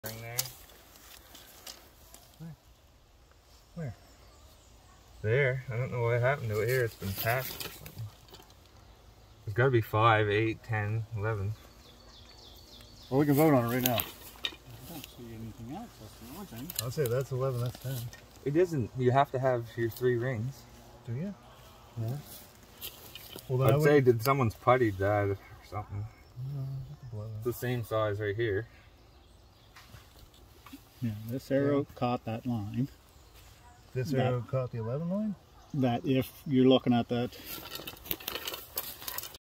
YOU make the CALL 10 or 11 3D Archery
Our group had a tough time making the call on the blue fletched arrow. Was it an 11, or was it a 10? the rings were VERY hard to see on the front of the Turkey. We ended up giving him an 11. What would YOU score it?